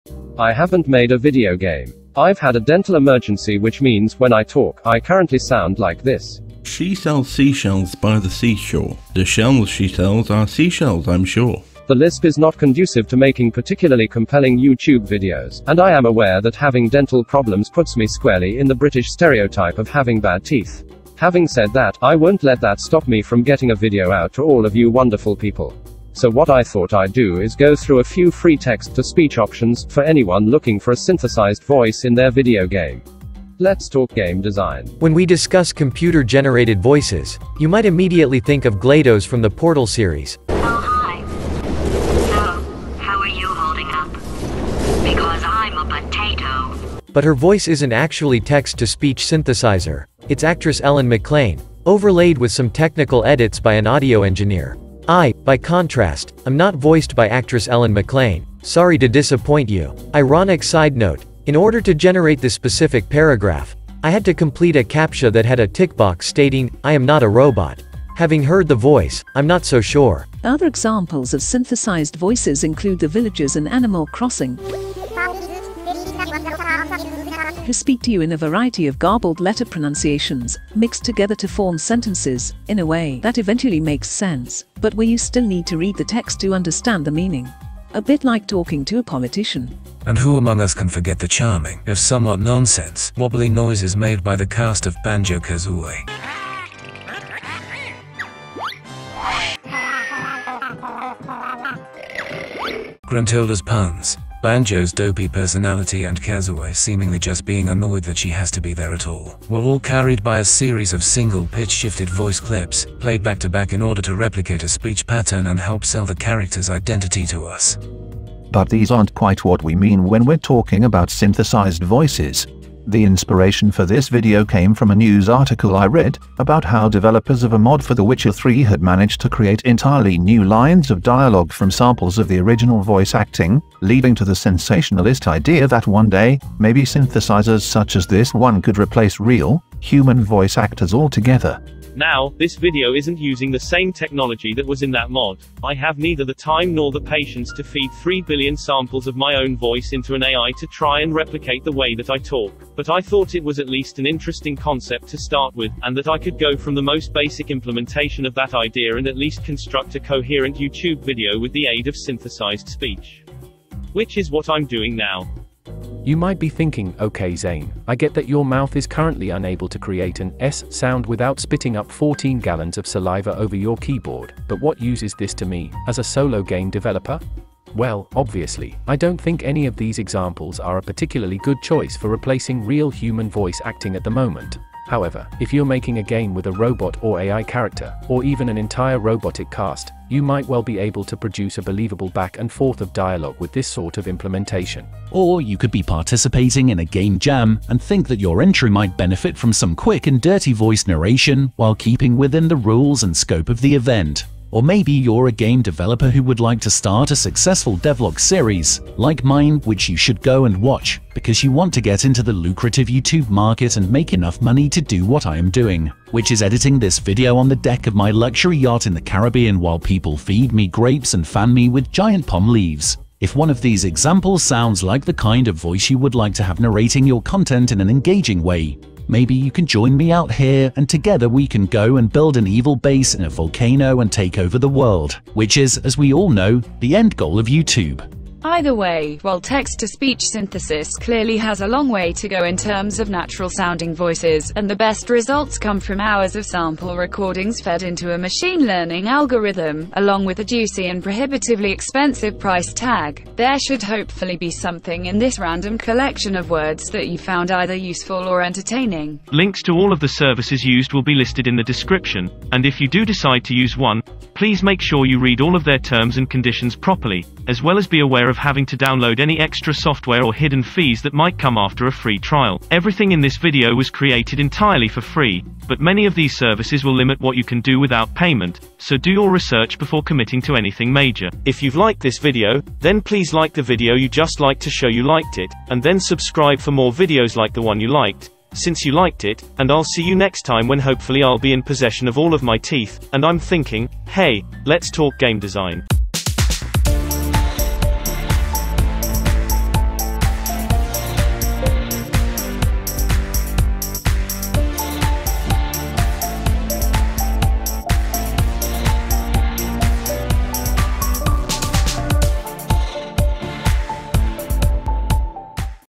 Best (Free) Text-To-Speech for YouTubers And Game Developers
Let's investigate Text-To-Speech, because I literally can't 'talk' game design right now. Here's 11 of the best (free) options that I found for text-to-speech, if you want to form a coherent Youtube video or add in some decent, semi-believable voice acting.

3. IBM - Watson Text To Speech Voices

WISHLIST QUICKLY, QUACKLEY! ON STEAM